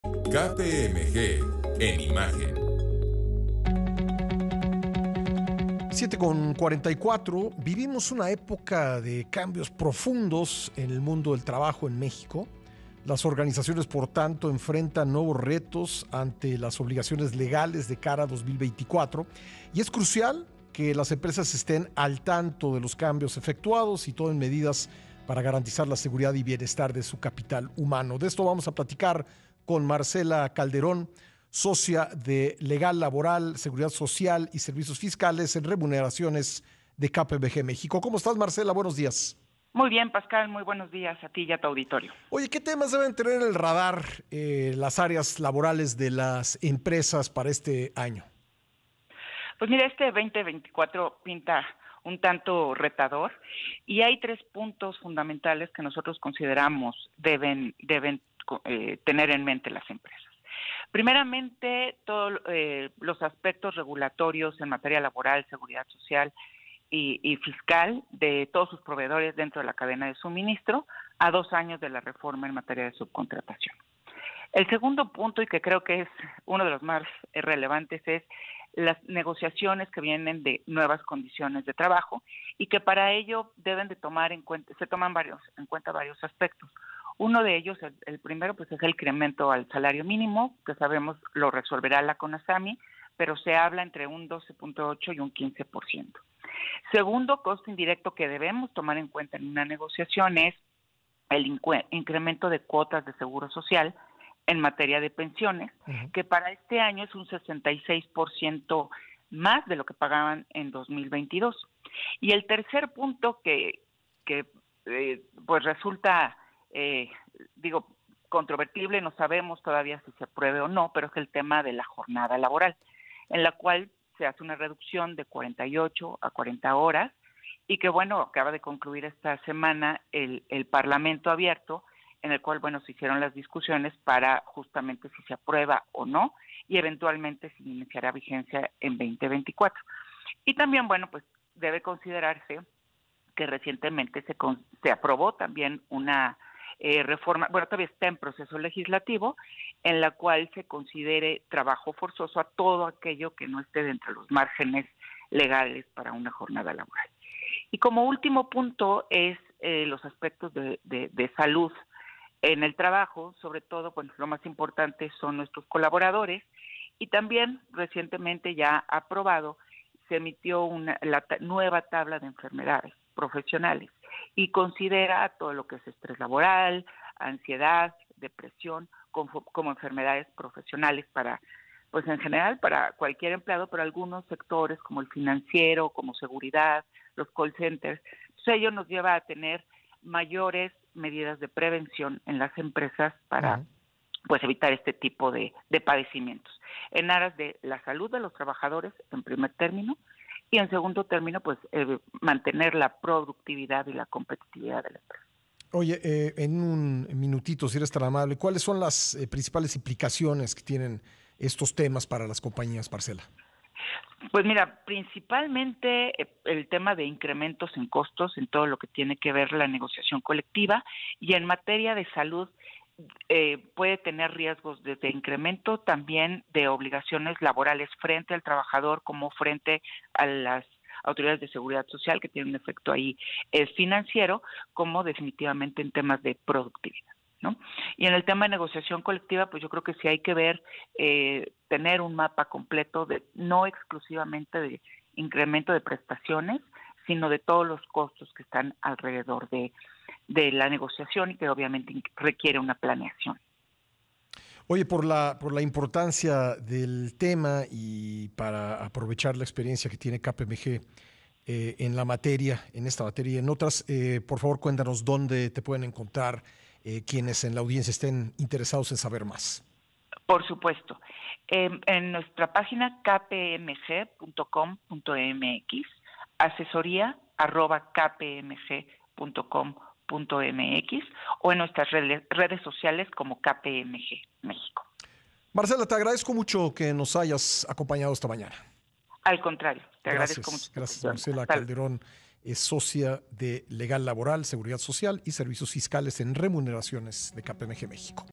KPMG | Cambios laborales que habrá en 2024
Marcela Calderón, Socia de Legal Laboral, Seguridad Social y Servicios Fiscales en Remuneraciones de KPMG México, habla en entrevista sobre los temas que deben estar en la agenda laboral de las empresas para 2024. Todos los detalles en el video.